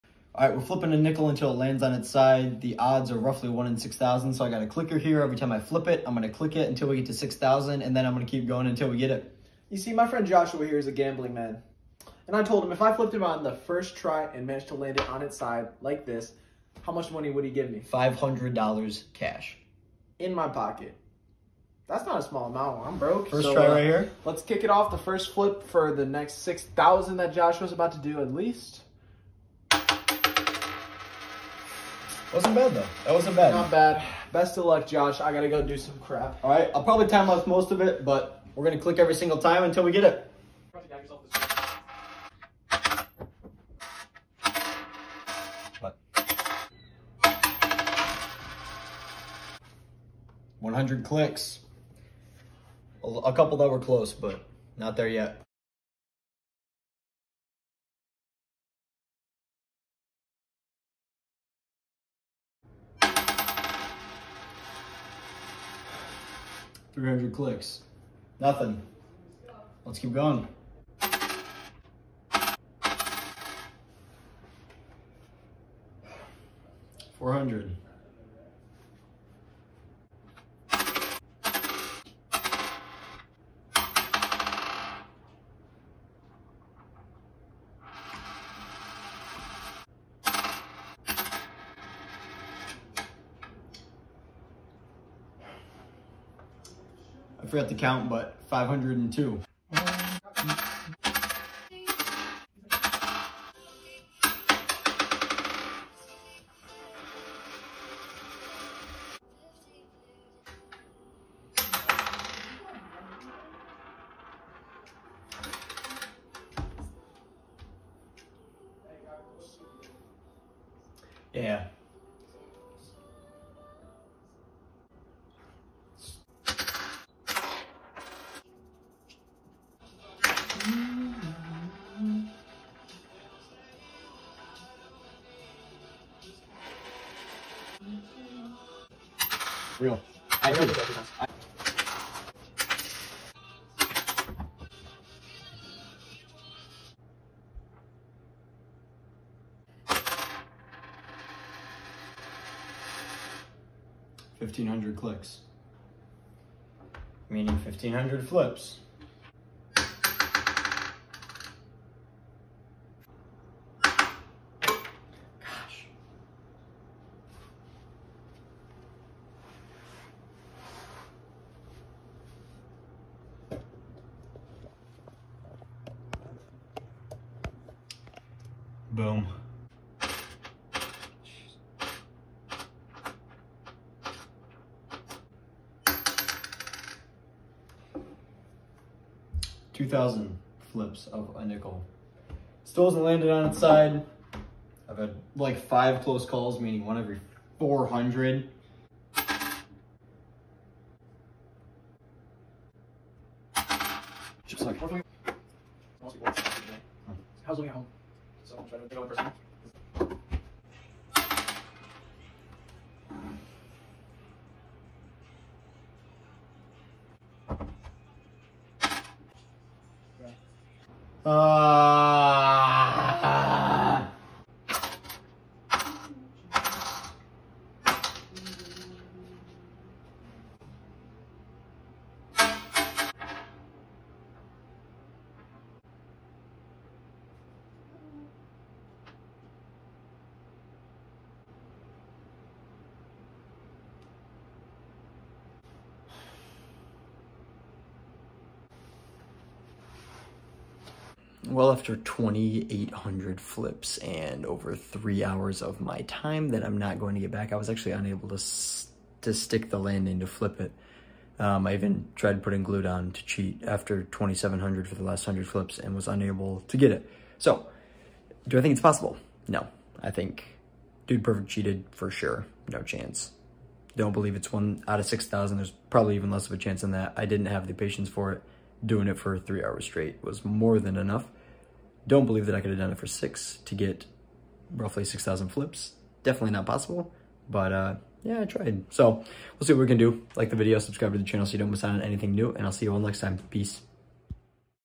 Trying to Flip a Nickel on its Side
This was honestly the hardest challenge I’ve ever done, and it's a real shame I was unable to land it... but feel free to check out the other videos within this playlist where I did complete the challenge!